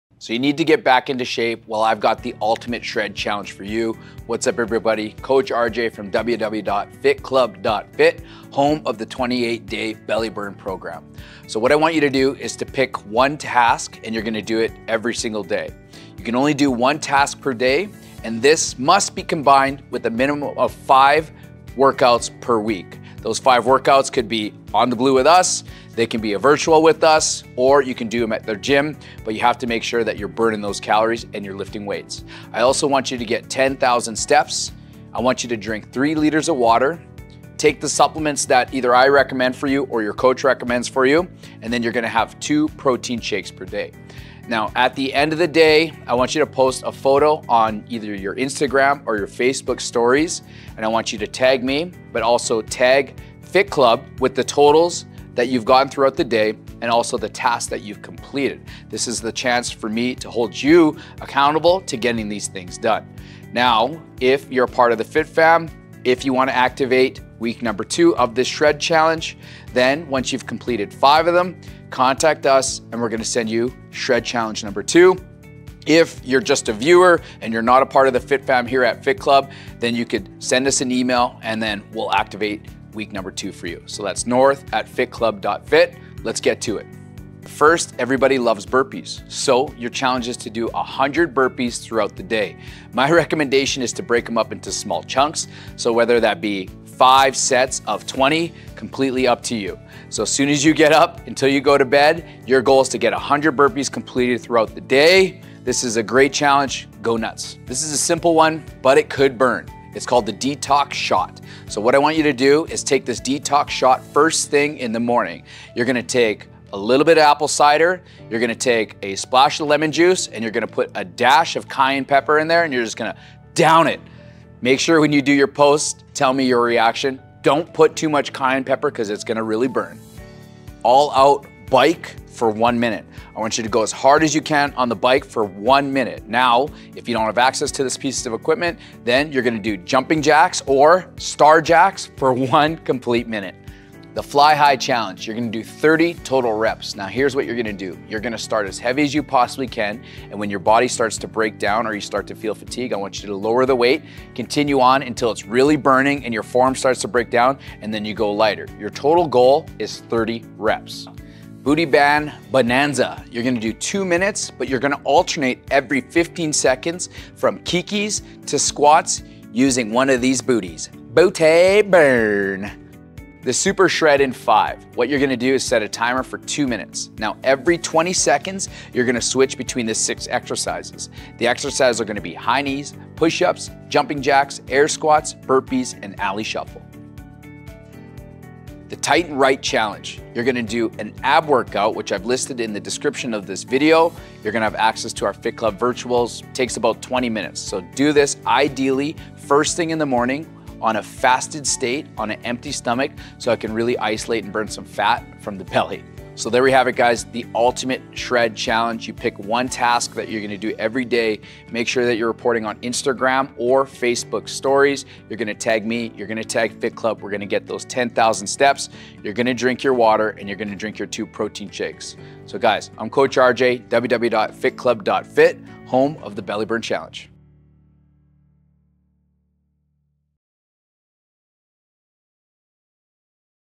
🔥 The Shred Challenge: WEEK 1🔥
How It Works:
Pick one task to do daily—only 1 task per day. Combine it with:

✅ 5 OTB workouts a week
🚶 10,000 steps daily
💧 3 liters of water
🛠️ Your supplements
🍹 2 protein shakes a day
At the end of the day, post a photo on your IG stories and tag me with your totals from the tasks.

To activate Week 2, I need to see your posts! Let’s GO!

WEEK 1 TASKS

100 Burpees Throughout the Day: From the moment you wake up until bedtime, it’s burpee day! Spread them out or go all-in at once. Go nuts! 😄

Detox Shot First Thing in the AM: Kickstart your morning with a 1 oz shot of apple cider vinegar (ACV), a splash of lemon juice, and a dash of cayenne pepper.

All-Out Air Bike x 1 min: No air bike? No problem! Do jumping jacks or star jacks for 1 min.

Fly High Challenge: 30 DB or cable flys, no breaks! Start heavy, go to failure, then drop to lighter weights. Feel that crazy pump!

Booty Band Bonanza: 2 mins, alternating every 15 seconds between KIKI’s and squats with a booty band. BOOTAY BURNNNN! 🍑🔥

Super Shred in 5: Set a timer for 2 mins. Every 20 seconds, switch exercises: High Knees, Push-ups, Jumping Jacks, Air Squats, Burpees, Ali Shuffle.

Tight and right Abs: Abs are a great active recovery workout.  Do this ideally first thing when you have no food in your stomach.
Don’t forget to check in for this workout in the Fit Fam.

Challenge Accepted? Let's crush this week together! 💪 Tag me in your IG stories, and don't miss out on Week 2!